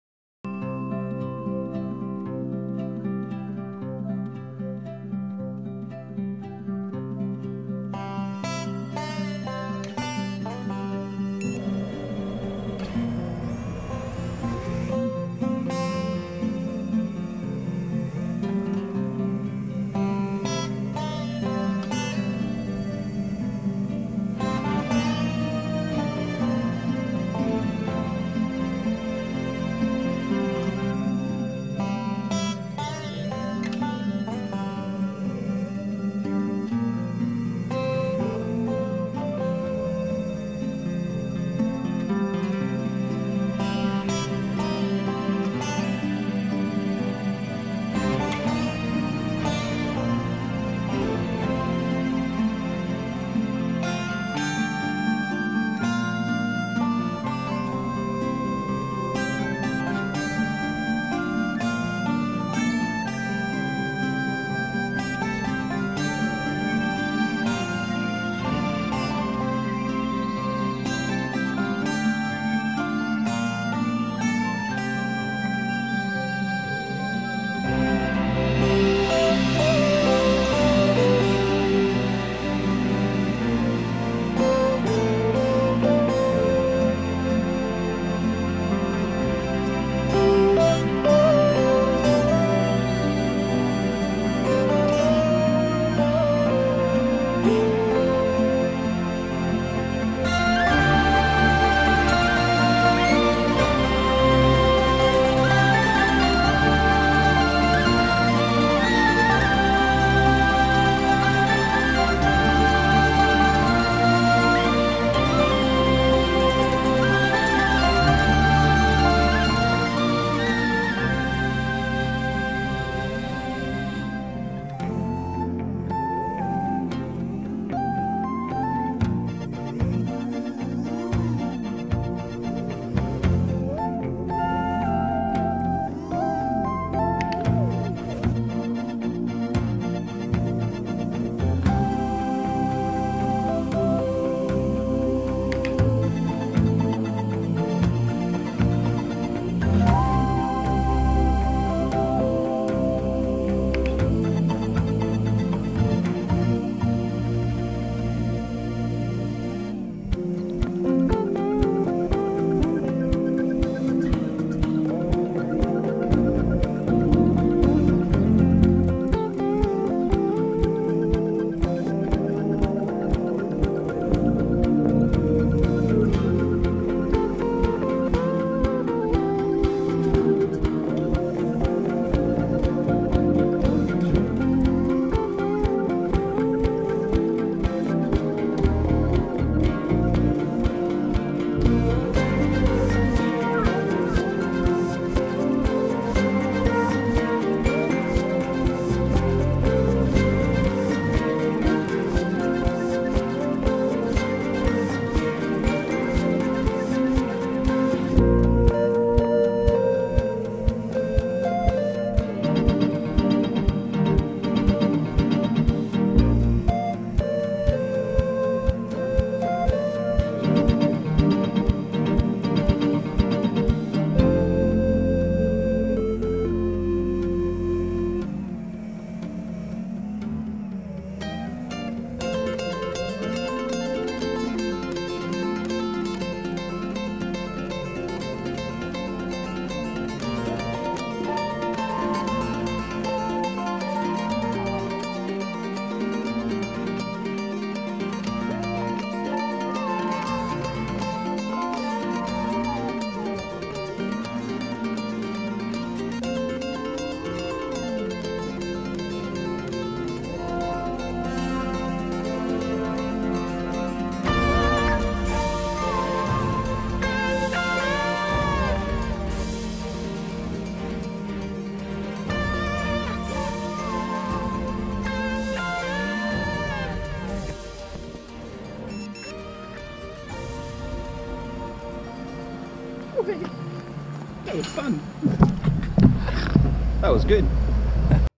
Cruise thru the park before work
I forgot how nice it is to have a quick fly before work on a nice summer morning..